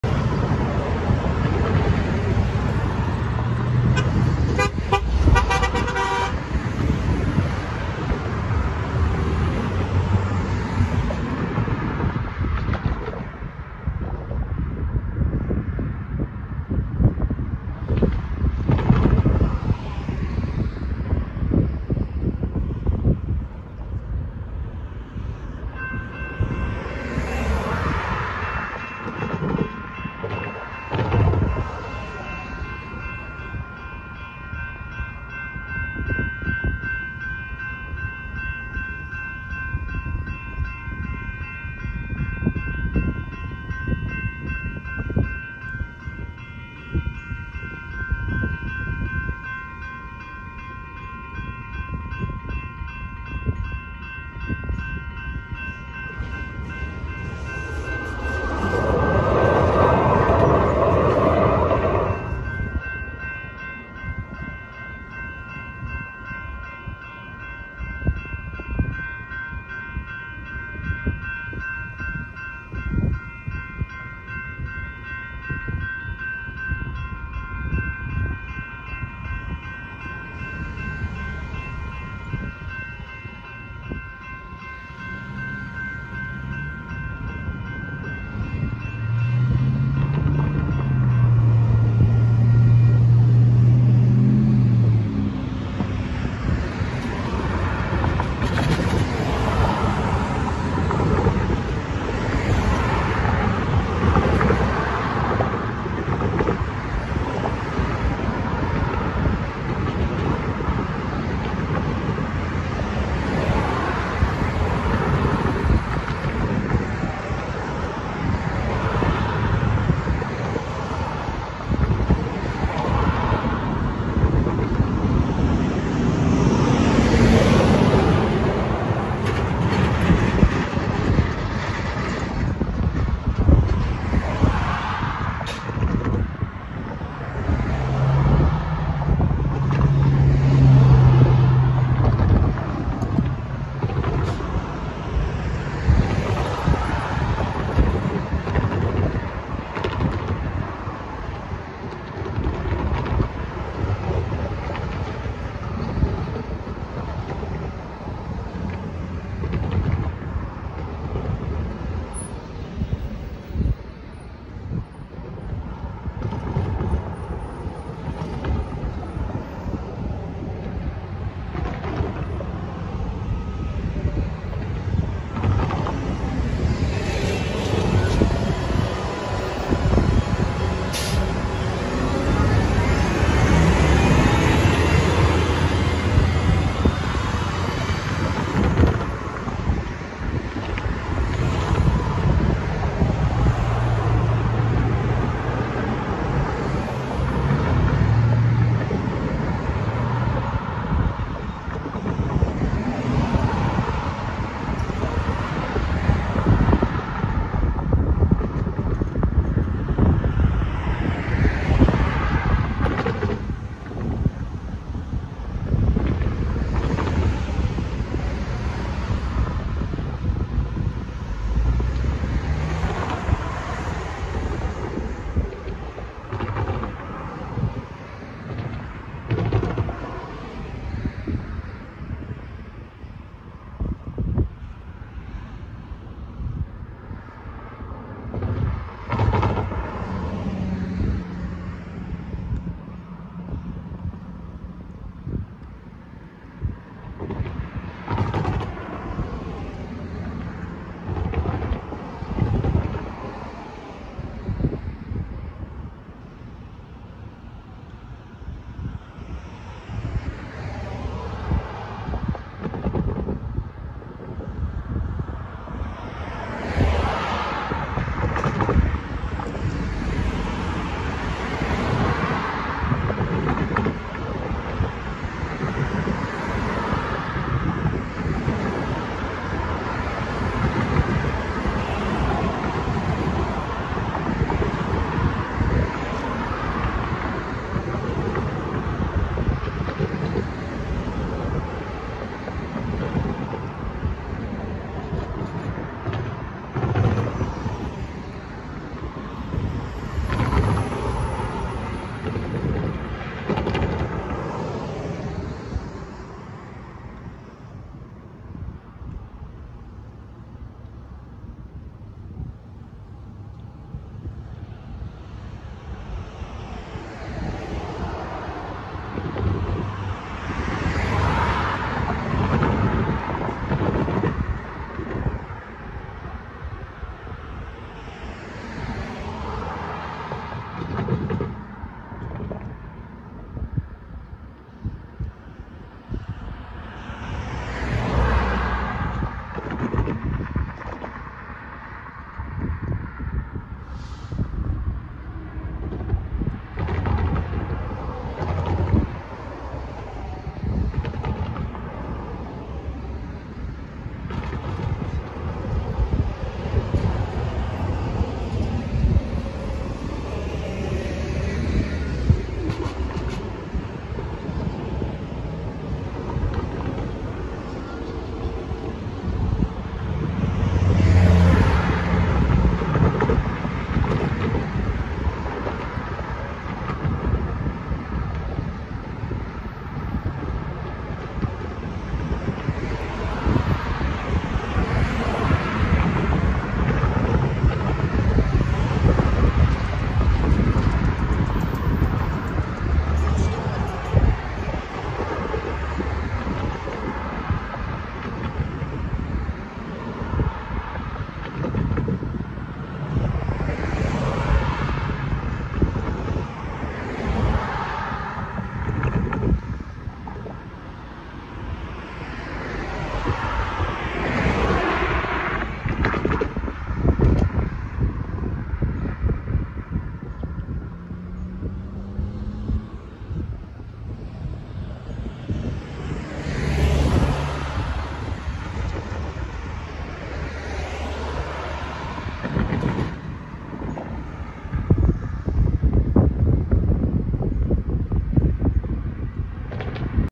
SACRT 116 - Meadowview Road Railroad Crossing, Sacramento CA
(Filmed August 17, 2023) On the way out my mom and I stopped by here at Meadowview Road where I got an inbound light rail. This was my last catch of the day. The close signal had its gate replaced after the previous one was damaged from the January storms earlier this year. This crossing is jointly owned by Sacramento Regional Transit and UPRR. The signals on this side are a SACRT mid-2010s install with changes to the gate and gate lights. The other side owned by UPRR is a mixed install with the left cantilever being a WP 1970s install with changes to its bell, lights including the light heads on its mast, and bracket/ junction boxes on the mast. The far gated mast is an early 2010s install with changes to its gate and gate lights. The far gate less signal is a UPRR mid 2010s install. There are also two yellow flasher warning signals for traffic not to stop in the tracks that only activate when traffic reaches the tracks from the intersection behind me. There are also two train blank out signals with one having a Siemens base and a 7 inch Safetran dwarf signal.

Sacramento Regional Transit - Blue Line - Inbound - 116, 125 Siemens Deuwag U2A

Ending of my videos explained: Here to explain the ending of my train videos!84 views13 hours ago
› chasefan...ChaseFan No. 9 (Merry Christmas ...